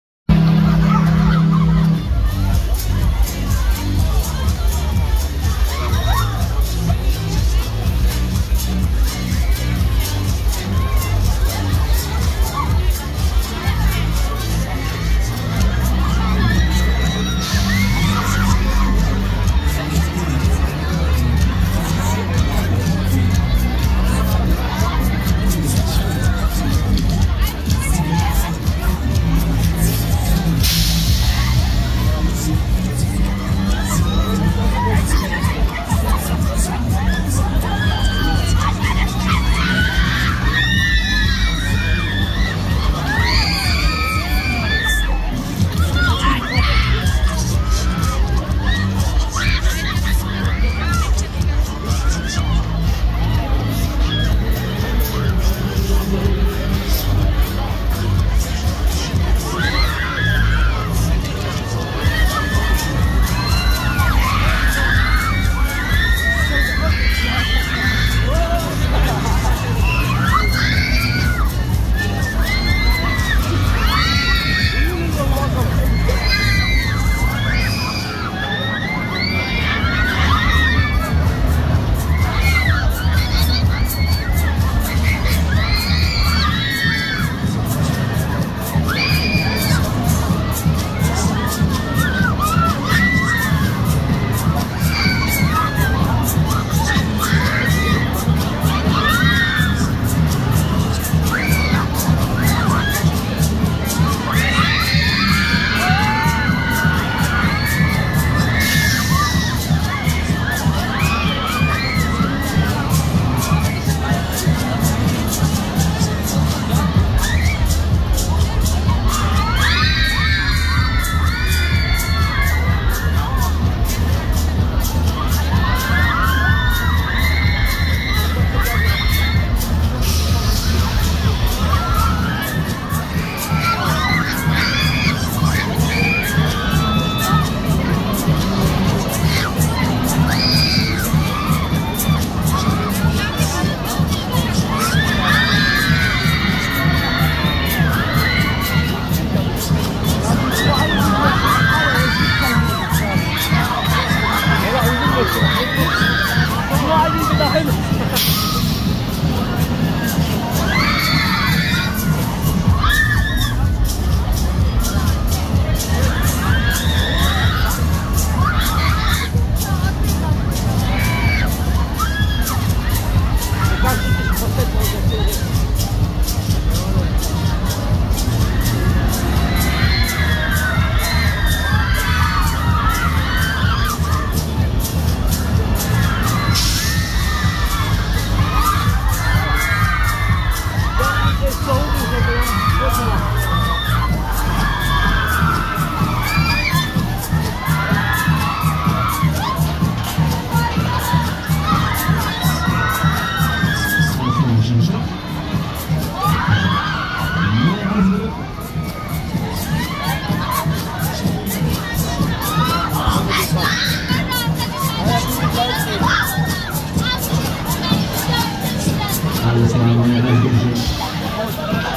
Revolution - Simoneit (Offride/POV)Video Family Wonderland Berlin 2023
Fahrpreis:6€